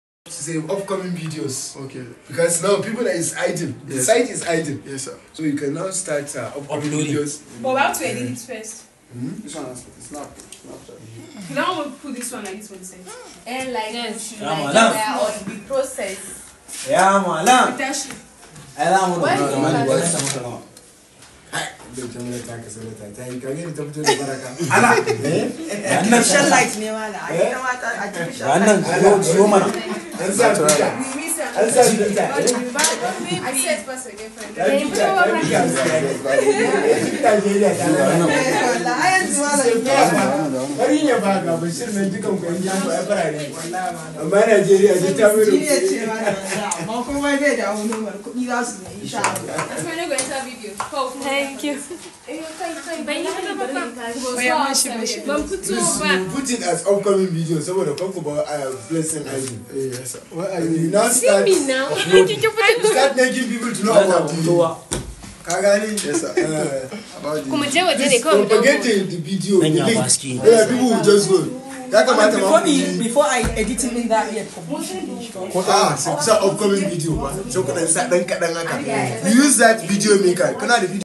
Kadpoly Scoop Squad preparing for their first interview
Kadpoly Scoop Squad preparing for their first interview with their Project Supervisor in person of Mal. Sanusi Rufai Buhari